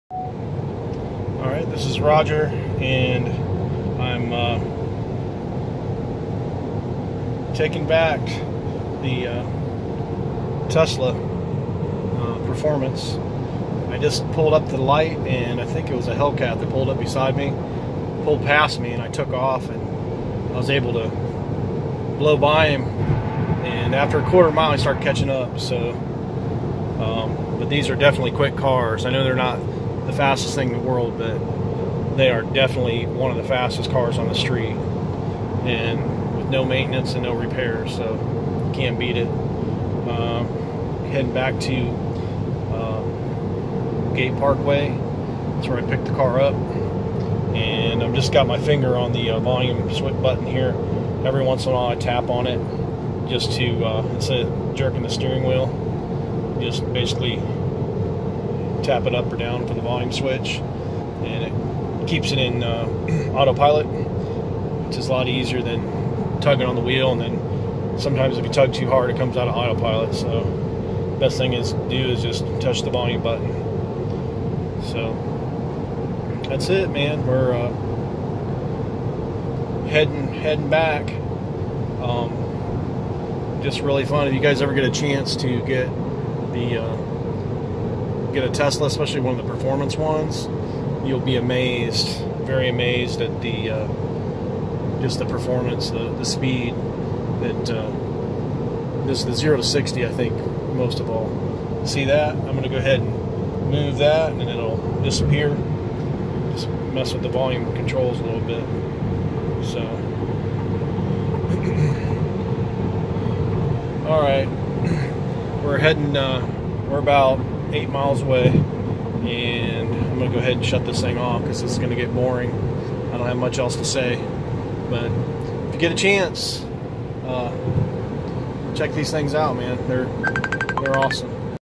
June 28, 2020 TAKING BACK TESLA MODEL 3 PERFORMANCE
ROGER TAKES BACK MODEL 3 PERFORMANCE AFTER A LONG FUN WEEKEND